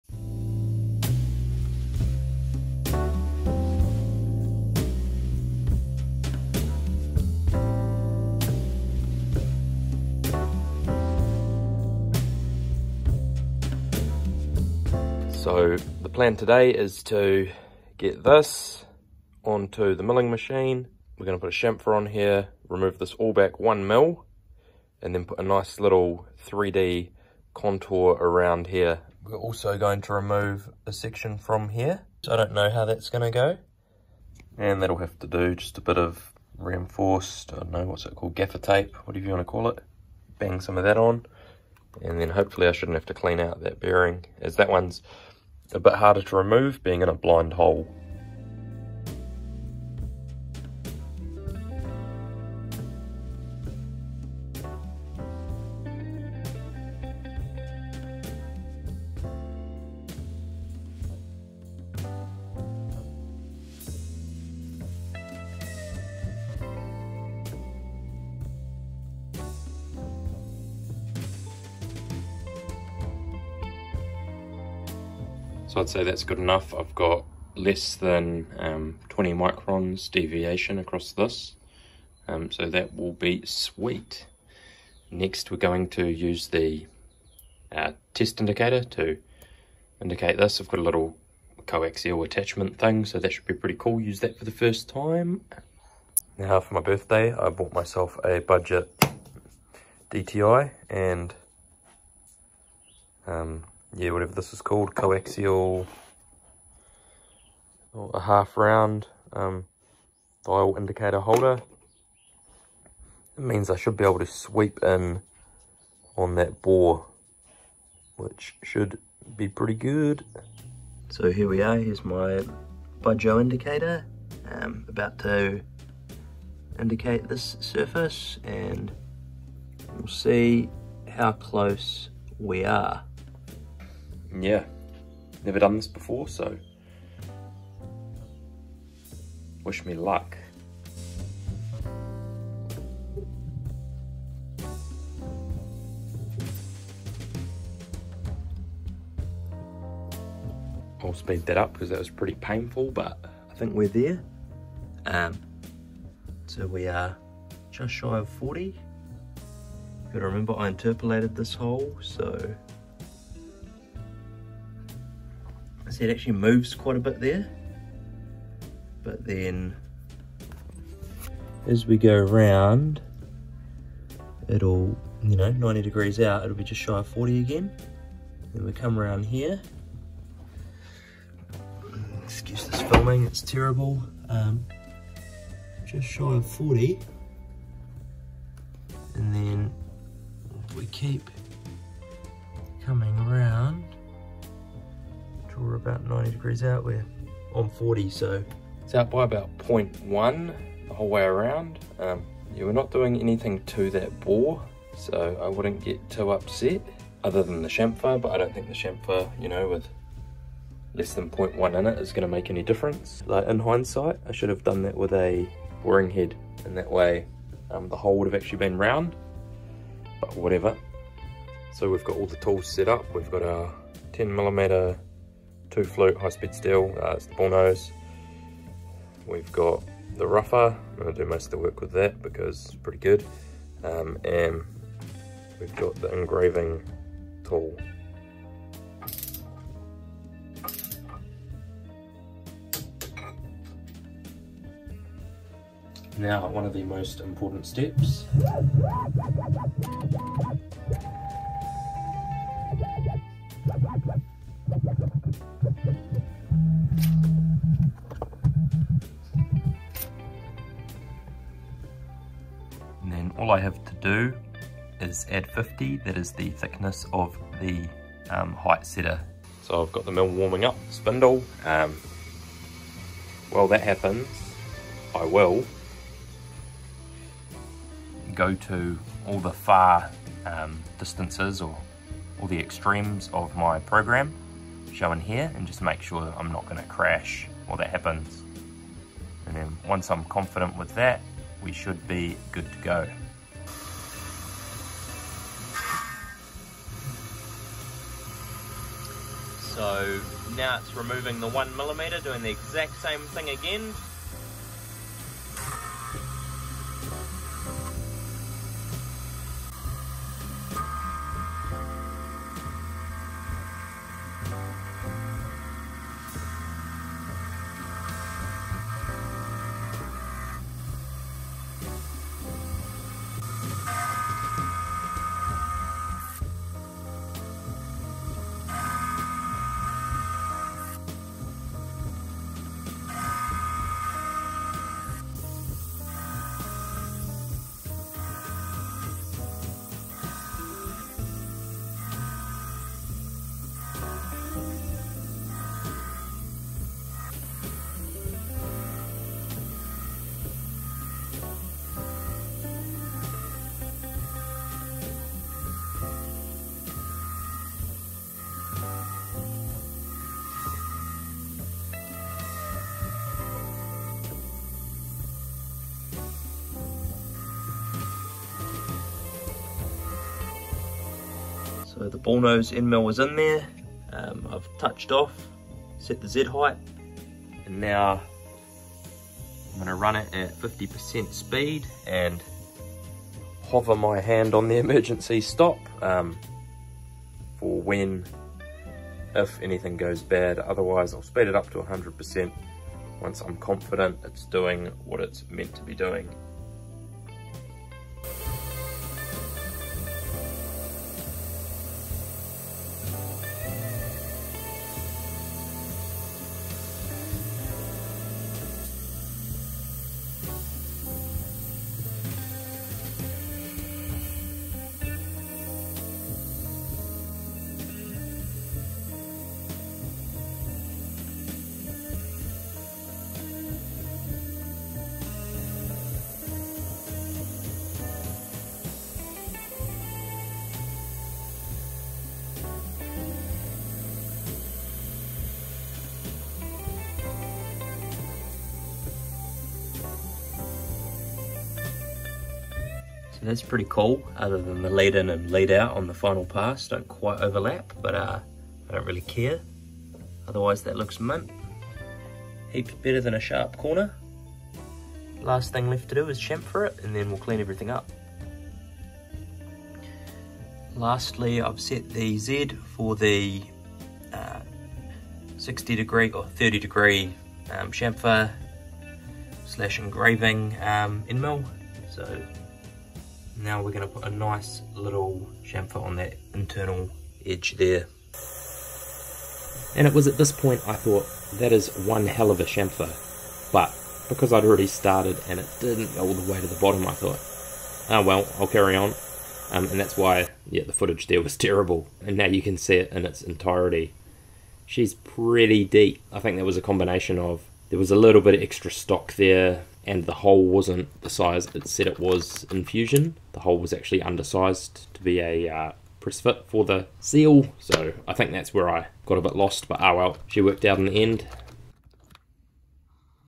Machining my Handcrafted V twin engine
In this update I fix my Home made V twin engines critical design error using my optimum CNC milling machine and fusion 360. Follow along as I take you through the set up and machining from start to finish.